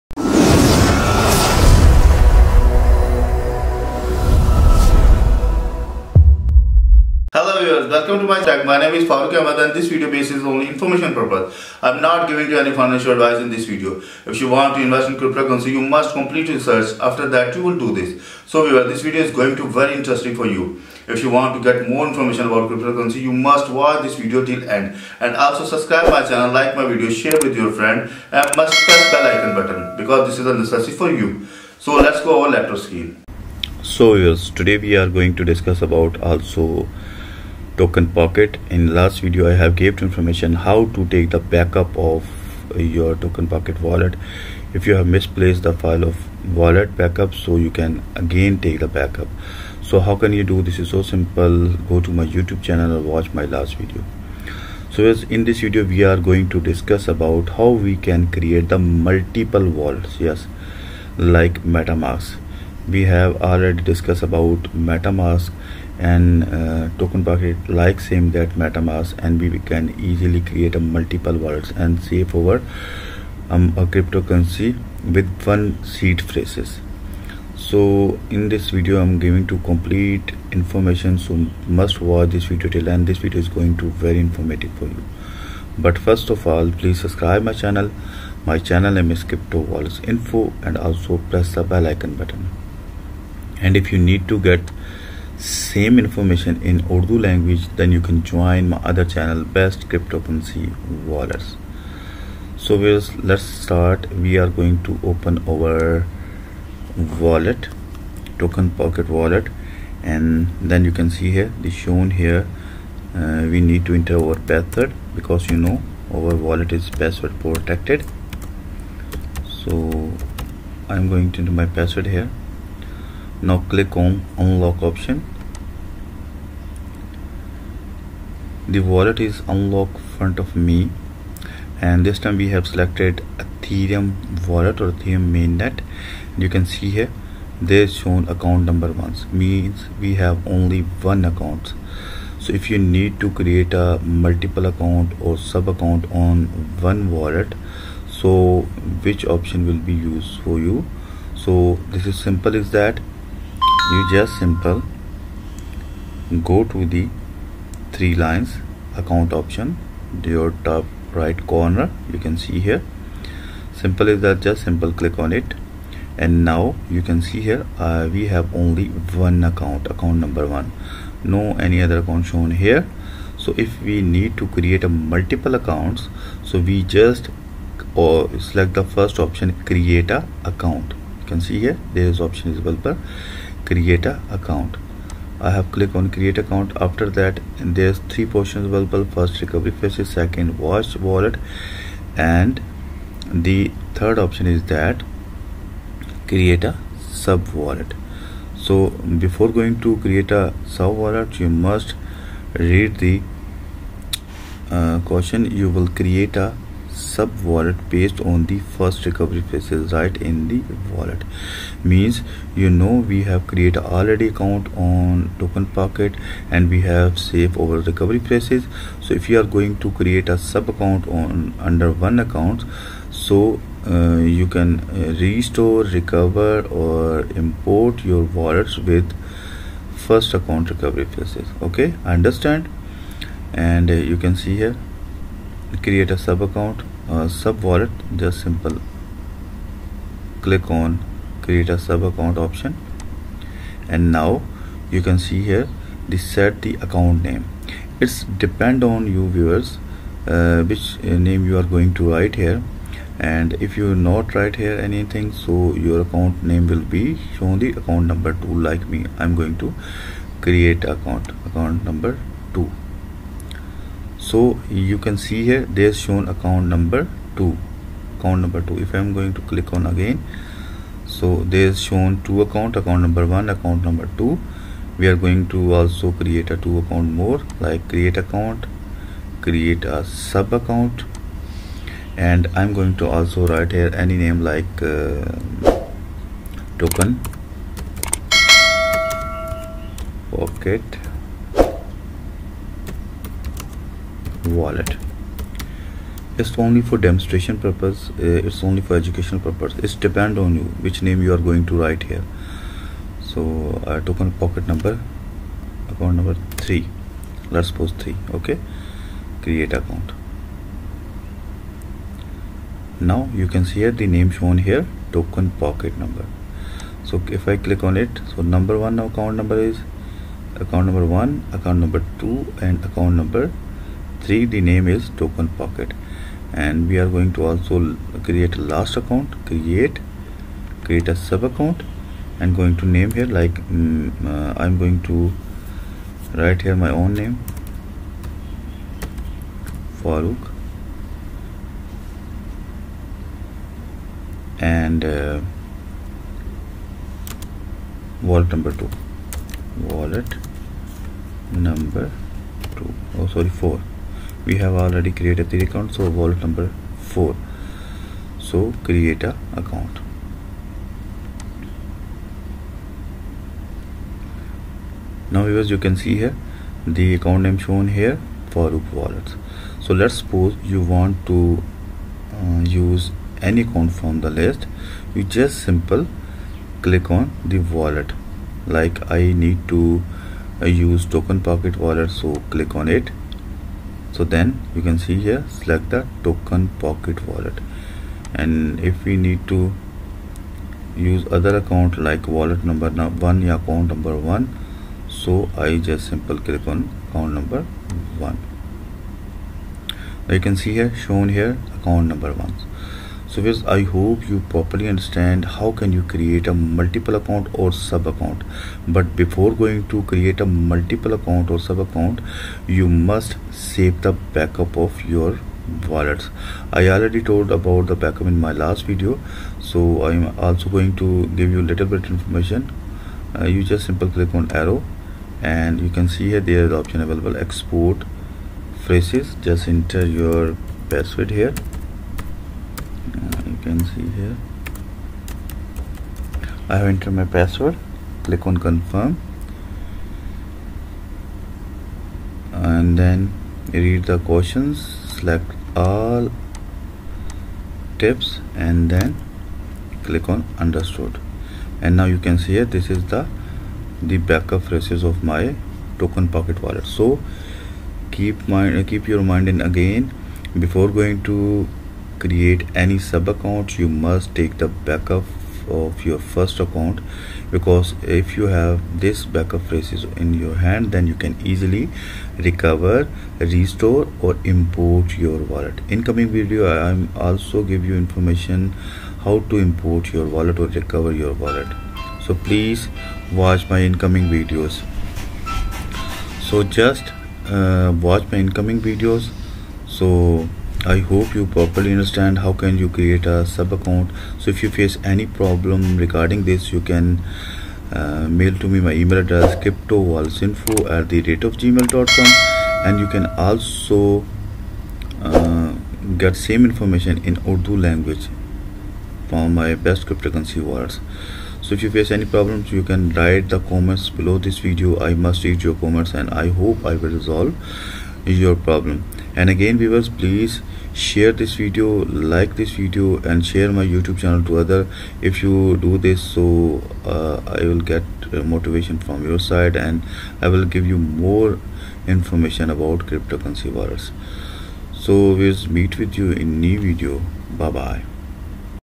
Steps to create multiple wallet on TokenPocket | Cryptocurrency Wallet | Bitcoin Wallet
How to Create Your Token Pocket wallet | Bitcoin Wallet | Cryptocurrency Wallet. TokenPocket is a multi-chain wallet which simply means users can create multiple accounts on the go and also hold/manage their Crypto assets whichever way they see fit.

That said, some users find this difficult to use and if you are part of this group, let’s quickly run a breakdown for you and ways to create more than one crypto wallet on TokenPocket be it on the same crypto or others.

TokenPocket Extension is a self-custody wallet, which means your secret recovery phrase is stored in your devices only. And it currently supports all the EVM-based chains, like Ethereum(ETH), Binance Smart Chain(BSC), Huobi Eco Chain(HECO), Polygon(Matic), Avalanche C-chain(AVAX), OKExChain, Fantom, and so on.

TokenPocket Extension strives to provide a security extension for users to discover the power of blockchain and make it no barriers for common users to join the blockchain world by providing the best possible experience for our users.